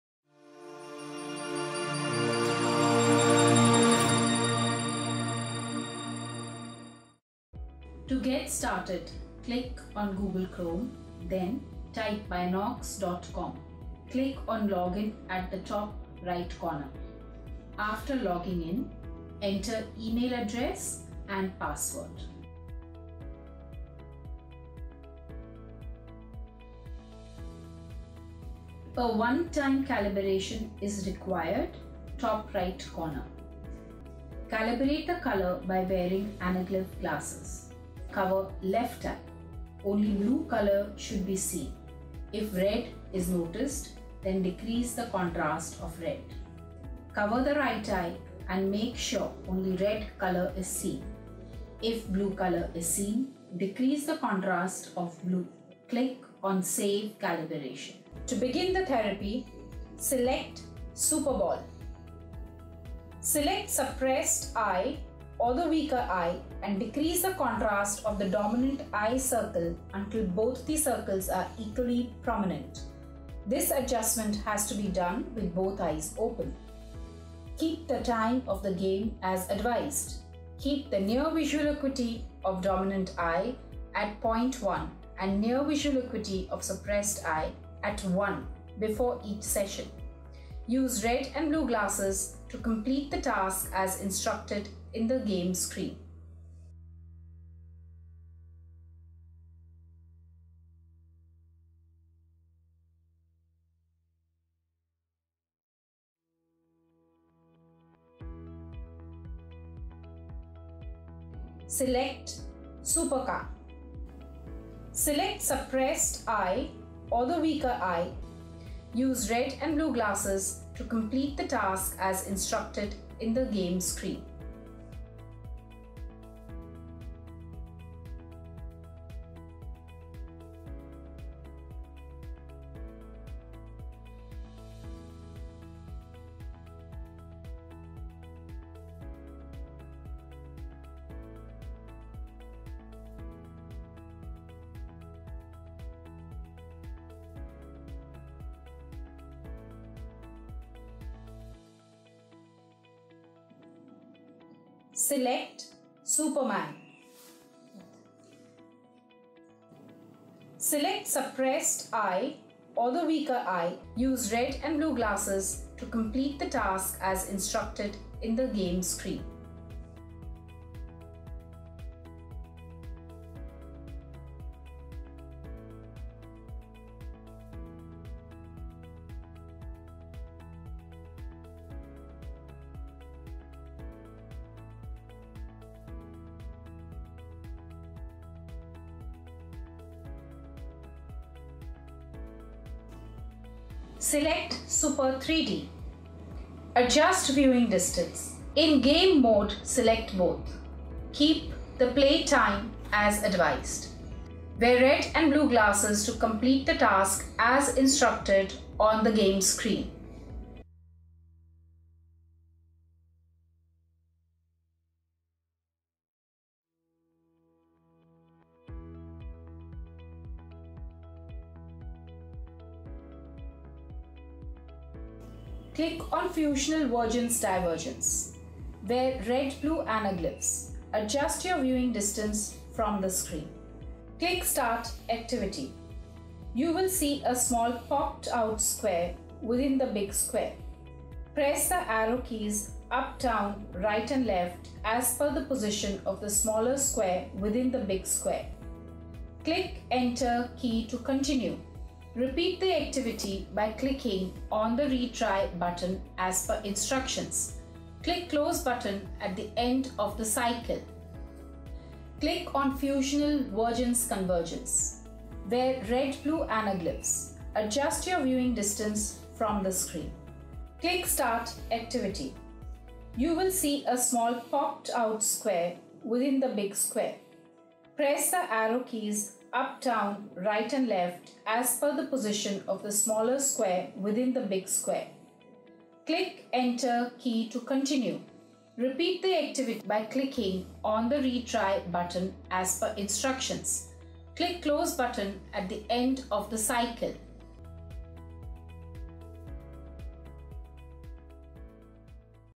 Bynocs Amblyopia Therapy I Lazy Eye Treatment I Dichoptic Games I
This tutorial is to demonstrate how to calibrate the screen before we start Bynocs amblyopia therapy. It also explains how to alter the contrast of the dominant eye to remove inter ocular suppression.  Please note that this therapy program should be carried out under eye care experts supervision only.